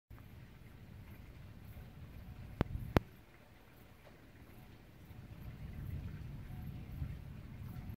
Blitzgewitter im Emmental
Am späten Abend vom 22. Juli im Emmental aufgenommenes Blitzgewitter. (Video David Bühlmann)